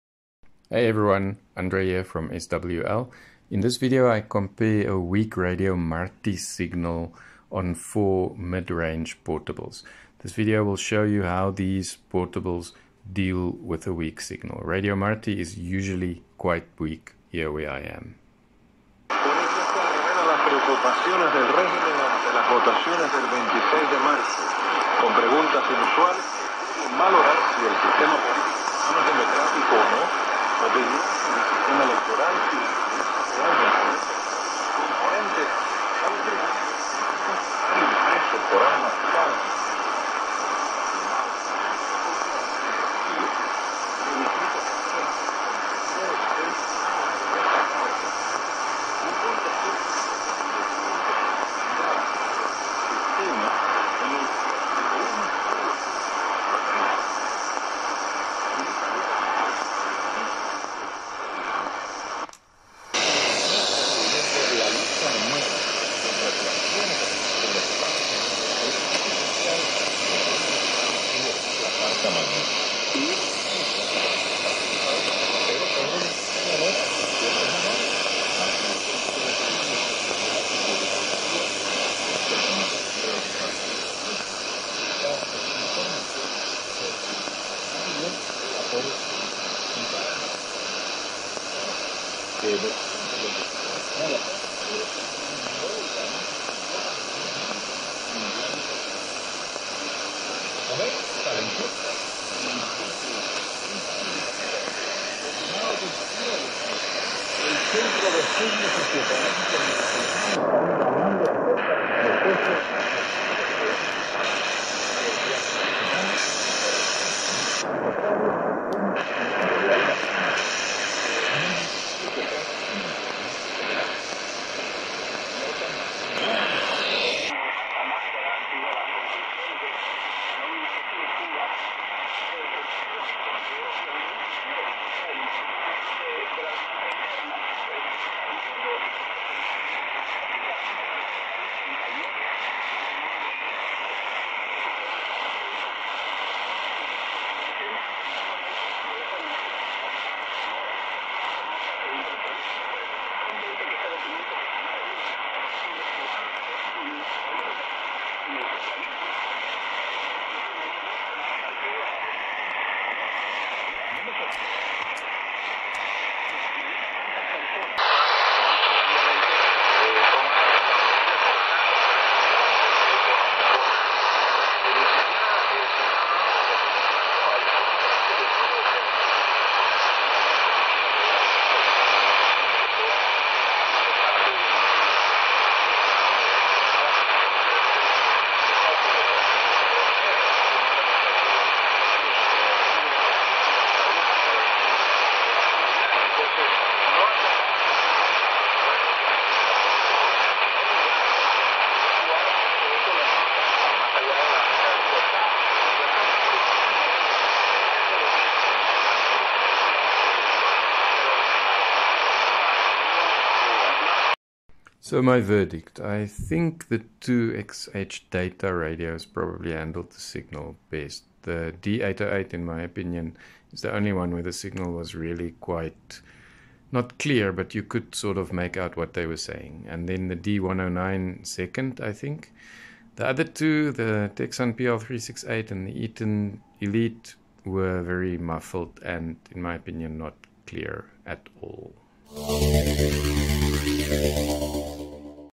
Weak Radio Marti 13820 kHz on XHDATA D-808, D-109, Tecsun PL-368 and Eton Elite Traveler
This video compares four ultralight radios on a weak signal from Radio Marti, 13820 kHz. Radio Marti is usually quite weak where I am in South Africa. I think a comparison of these four radios on a weak signal really shows what each radio can do. Recorded at my usual QTH location of KG33XV in South Africa, using the same 7 metre, 23 feet wire on each radio.

The four radios I chose for the comparison are all mid-range radios, ranging in price from about $50 to $80. The D-109 is currently available on the XHDATA website at $31, a special introductory price. The regular price is $50.

00:00 Introduction
00:19 XHDATA D-808
01:02 XHDATA D-109
02:07 Eton Elite Traveler
02:46 Tecsun PL-368
03:34 My verdict